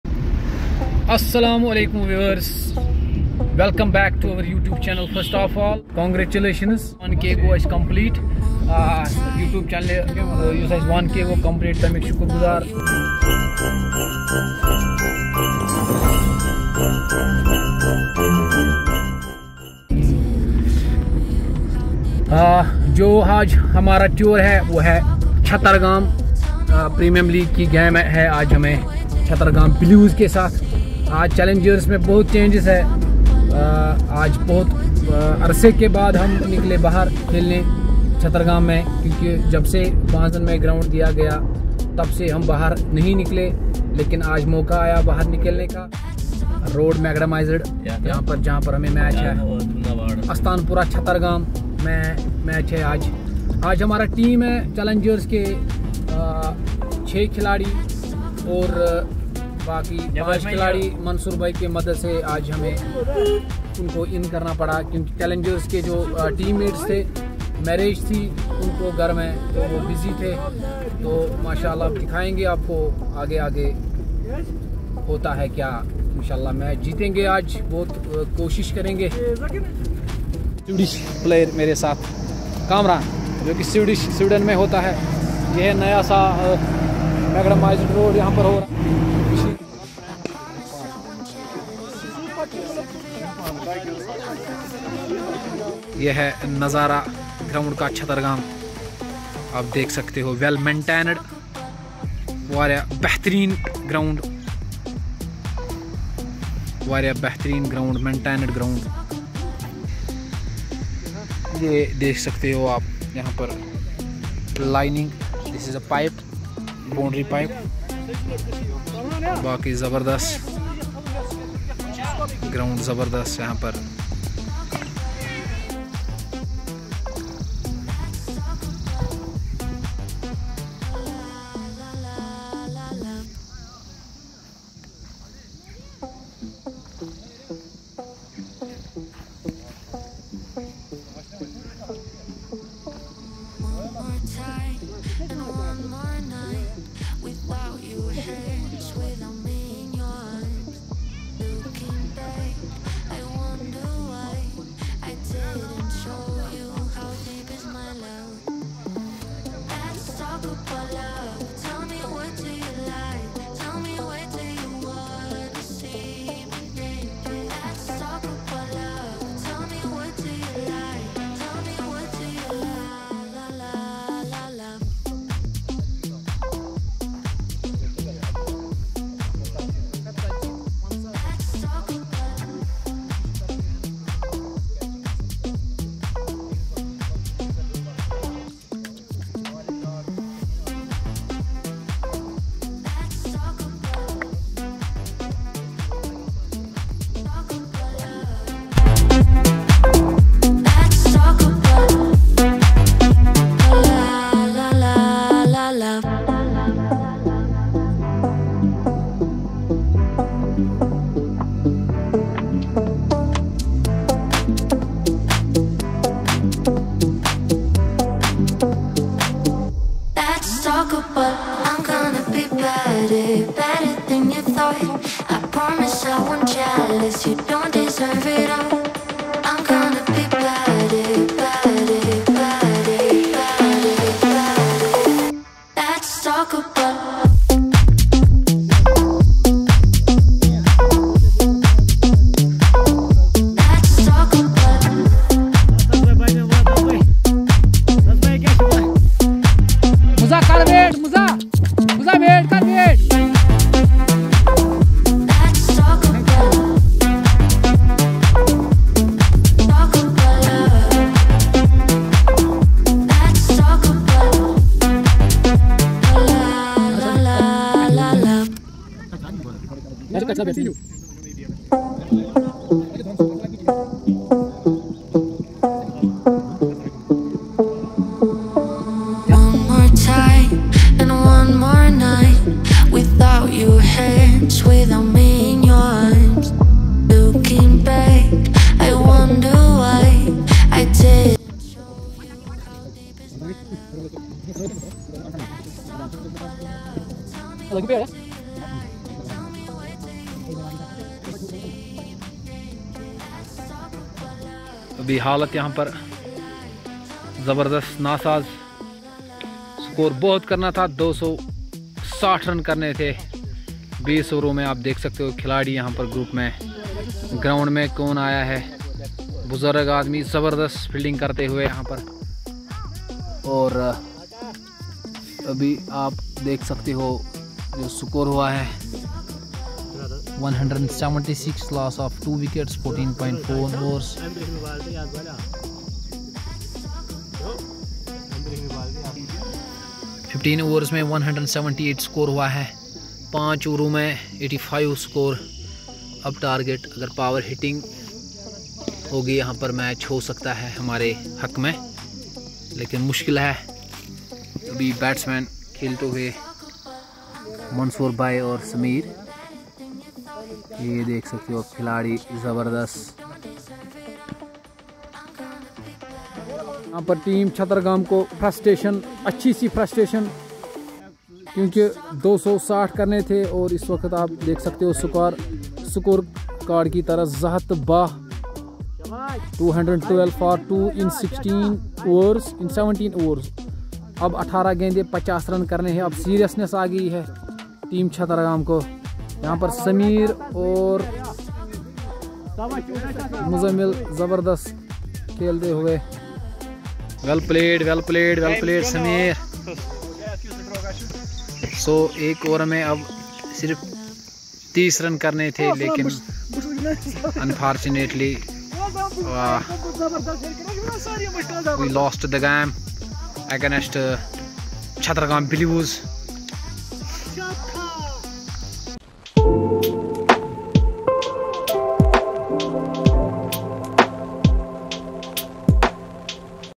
Cricket Tour to Chattergam Good Wicket Vlog No 25 2023
it was batting track we decided to field first then they set huge target 260 then in reply Sameer played a Good knock 92 Runs then we lost with 20 runs only